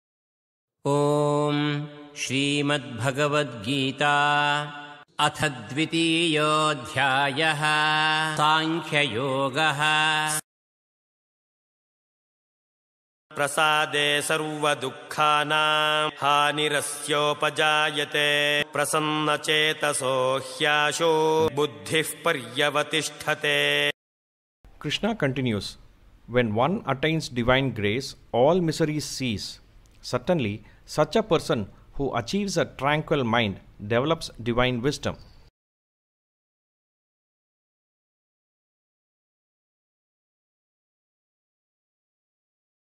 SRIMAD BHAGAVAD GITA - Chapter 2 - Verse 65
atha dvitīyo’dhyāyaḥ- sankhaya yogah
Chapter no 2 - Verse 65

prasāde sarvaduḥkhānāṃ hānirasyopajāyate |
prasannacetaso hyāśu buddhiḥ paryavatiṣṭhati || 2.65 ||

Krishna continues “When one attains divine grace, all miseries cease. Certainly, such a person who achieves a tranquil mind develops divine wisdom.”